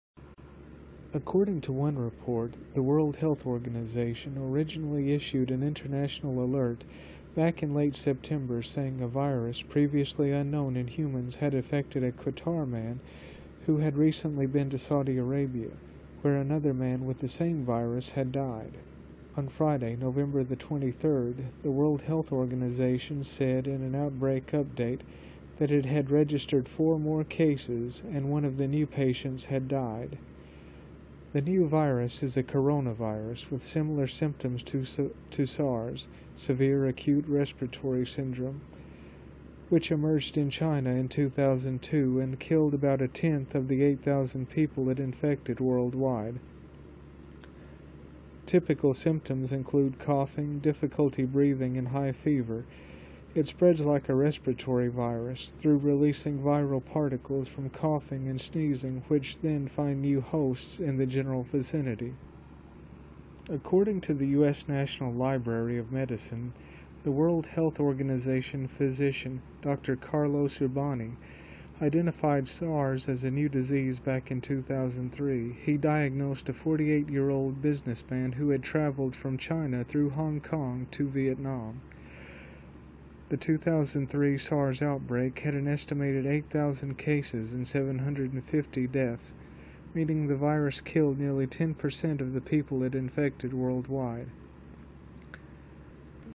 SARS-like virus in Saudi Arabia
The new strain is called coronavirus and has many of the same symptoms of Severe Acute Respiratory Syndrome (SARS), which include coughing, fever, and difficulty breathing. WHO issued an alert in late September announcing a Qatari man who had recently travelled to Saudi Arabia, was infected with the virus.